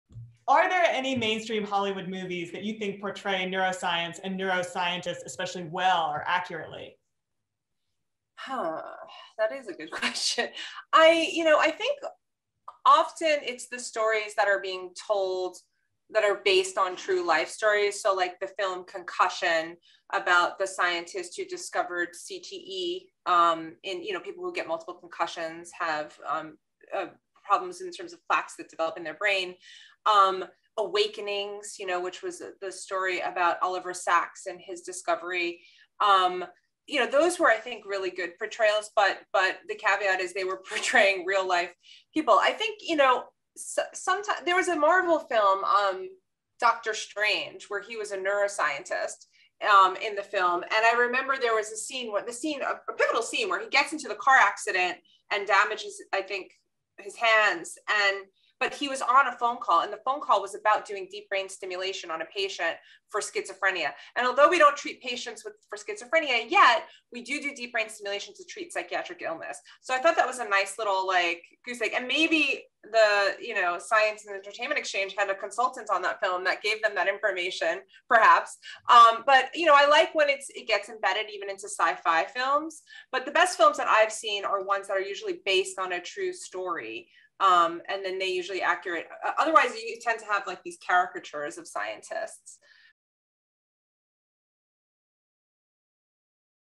ASK A SCIENTIST...about the portrayal of neuroscience in movies
Screenwriter Geneva Robertson-Dworet has a conversation with neuroscientist and Clinical Psychologist Heather Berlin about how neuroscience is depicted in movies.